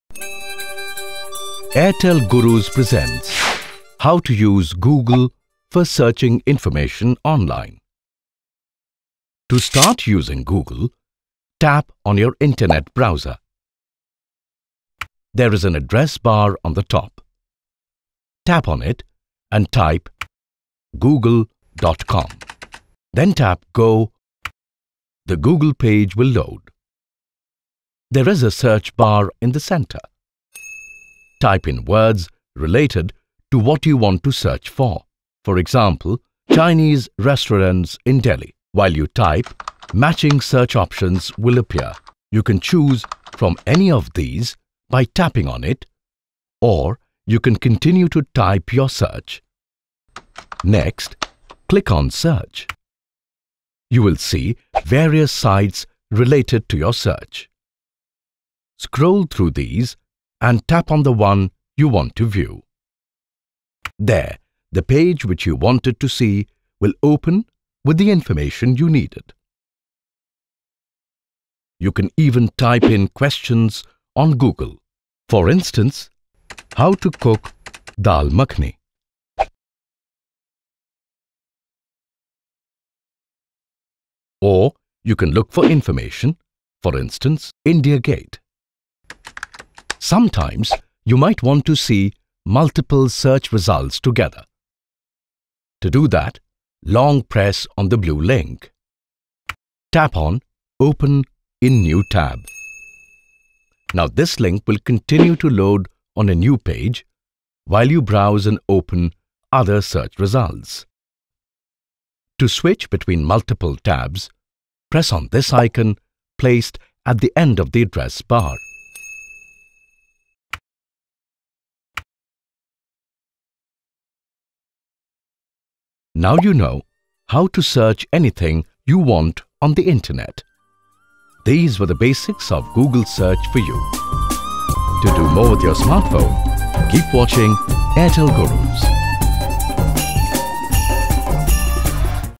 How to search using Google on your Android smartphone? (English)
"This airtel gurus video will tell you how to search using Google on your android smartphone.

You will learn how to use Google to search for information, news and much more.